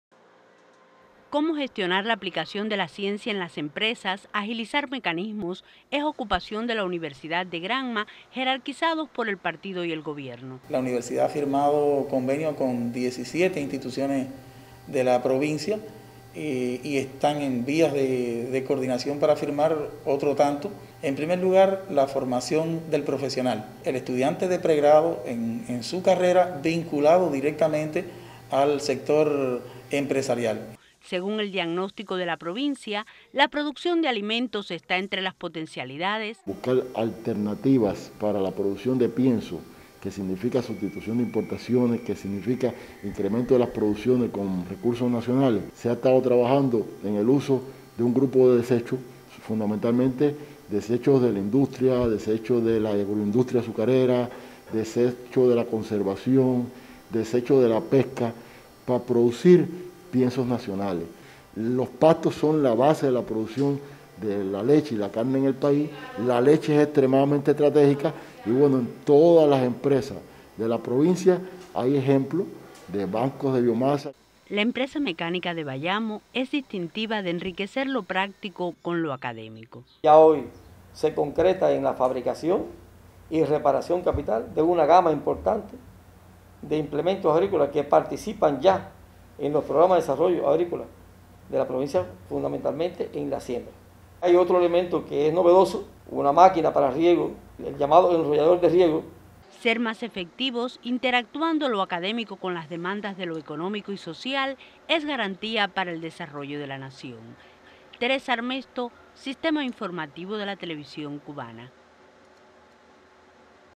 Vínculo universidad– empresa en Granma aporta proyectos para el desarrollo económico de la provincia
El vínculo de la  Universidad  de  Granma con la ciencia se expresa en unos 17 convenios  con empresas del territorio. Distinguen proyectos  relacionados con la producción de alimentos y el turismo. A ello se suma el impacto económico de las investigaciones aplicadas en pos del desarrollo agrícola, justo en en esta provincia con potencialidades para convertirse en el principal polo productivo del oriente cubano